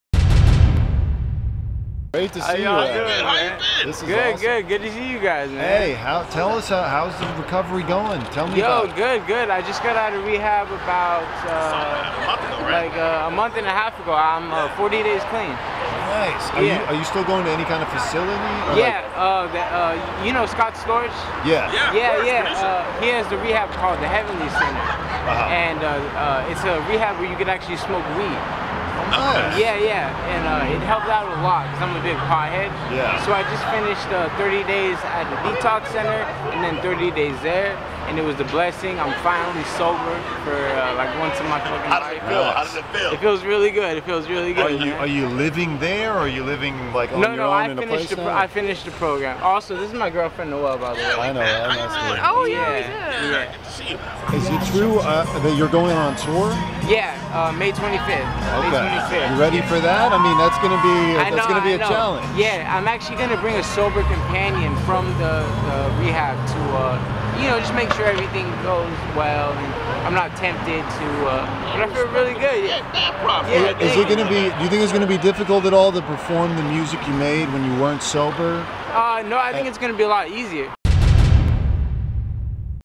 Lil Xan Says He's Still Clean, Bringing Sober Companion on Tour | TMZ Hip Hop
Lil Xan is not only doing much better, but he's taking extra precautions to make sure he stays sober when he returns to life on the road.

We caught up with the rapper this weekend at BOA Steakhouse ... where he told us about his progress and how he plans to stay on track in the future.